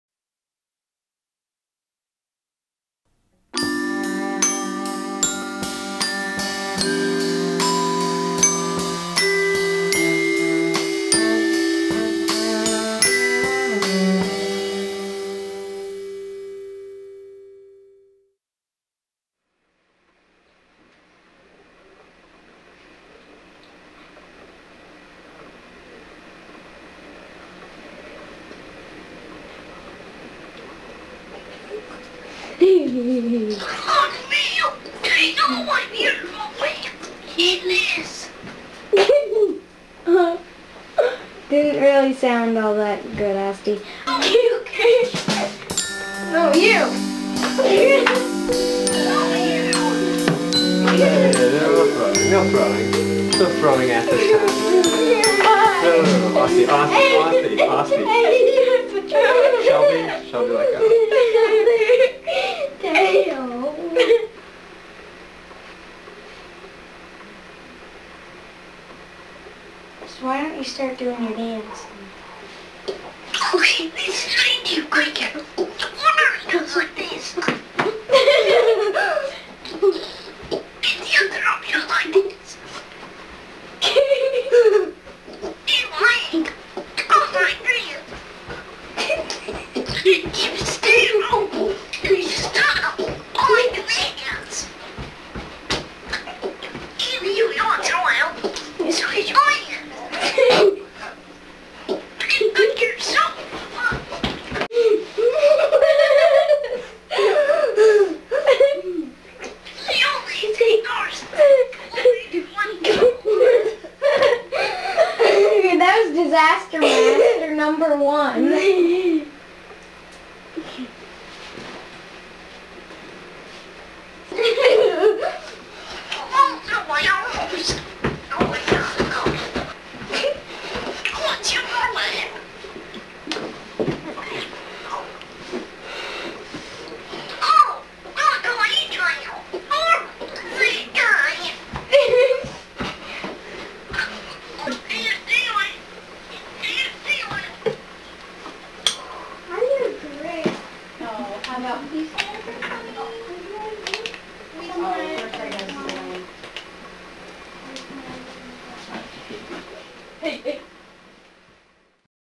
Gleeger - subtitled version
Kids talking in funny voices!
Subtitled for the confused!